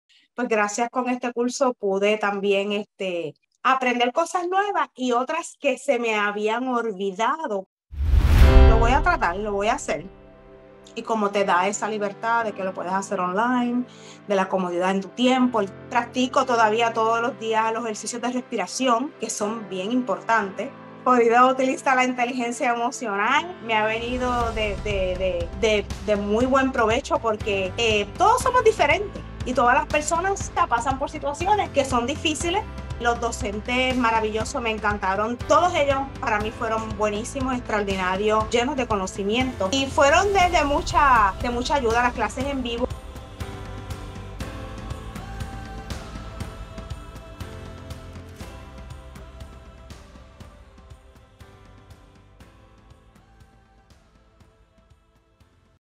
Aprende Institute Reviews | Yolanda Ortiz
Yolanda ha decidido mejorar su vida personal y profesional, y por ello ha realizado el Diplomado en Inteligencia Emocional y Psicología Positiva. Conoce su historia y mejora tú también tu estilo de vida con Aprende Institute.